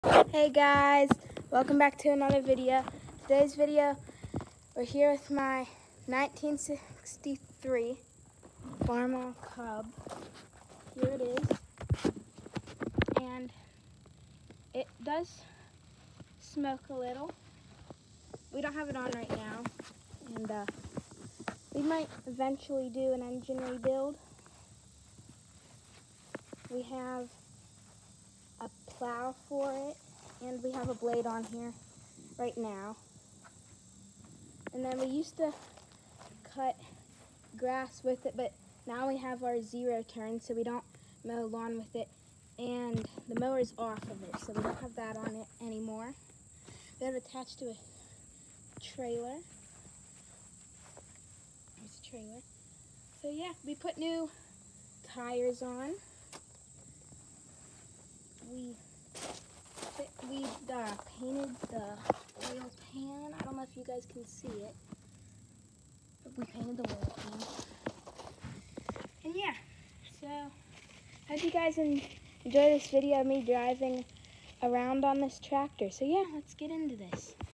Farmall Cub Walk Around
Talk about our 1963 Farmall cub, bought from original owner and maintained as needed with 80% of the tractor still being in its original work clothes.